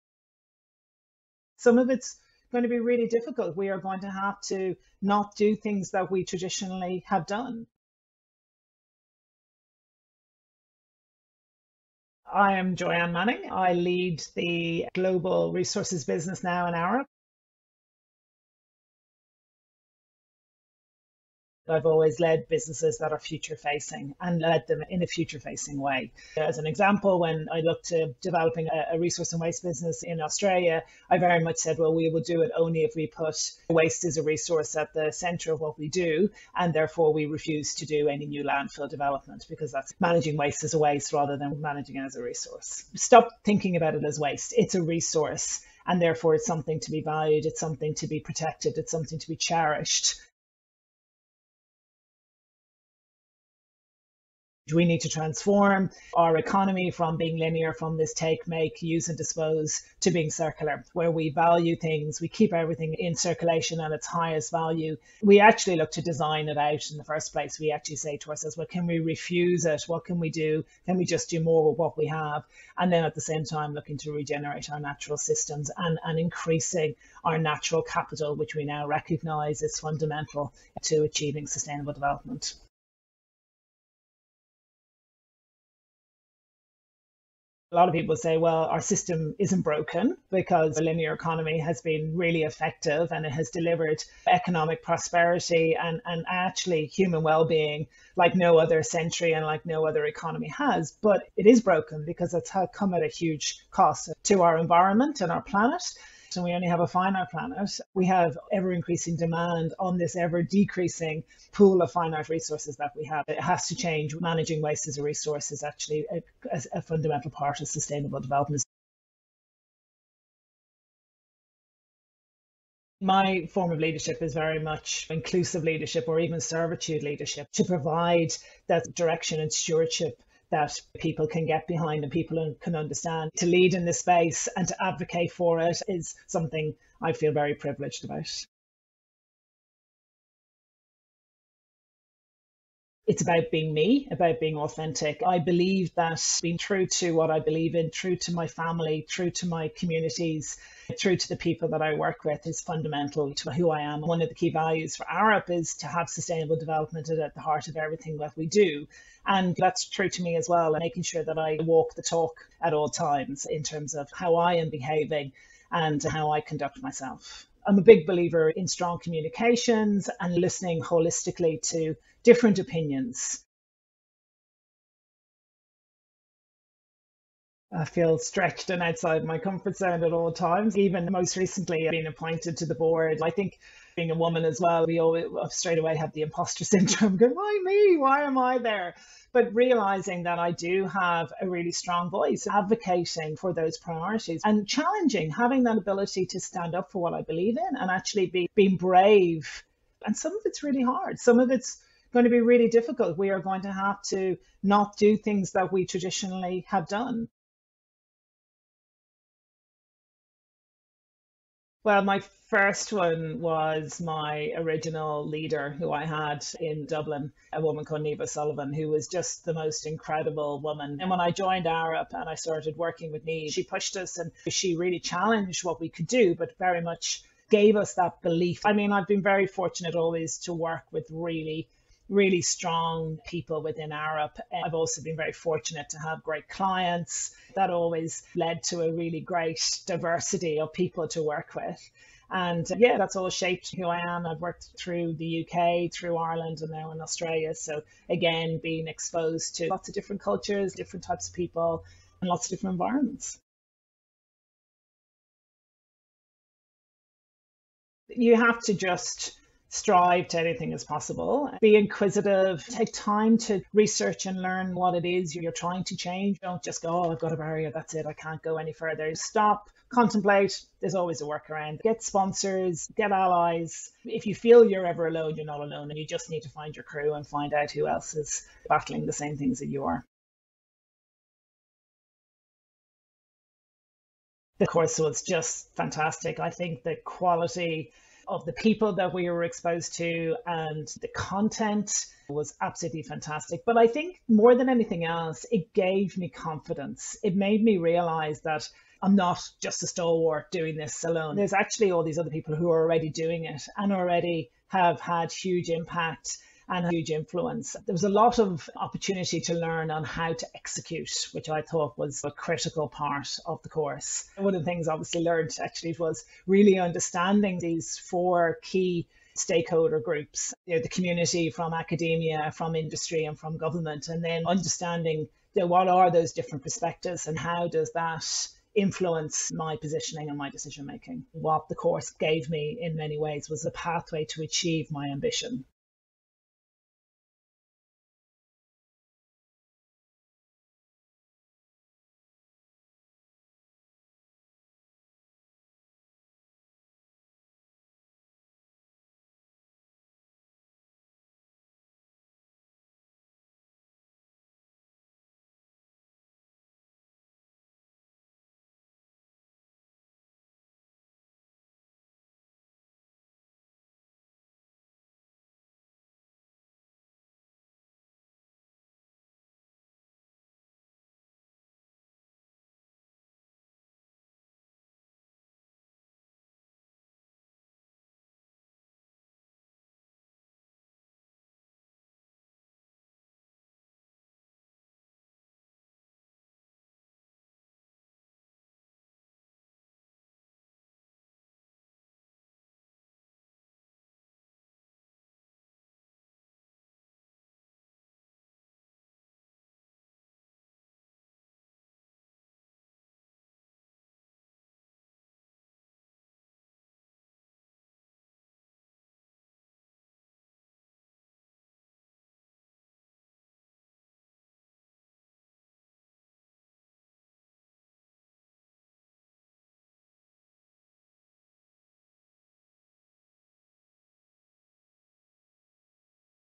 CISL Impact story - Joyanne Manning
We asked members of the CISL Network to share their stories of personal leadership and positive impact. Their stories span industries, continents, and cross-cutting themes from across the UN's Sustainable Development Goals.

CISL develops leadership and solutions for a sustainable economy. For people, climate, and nature.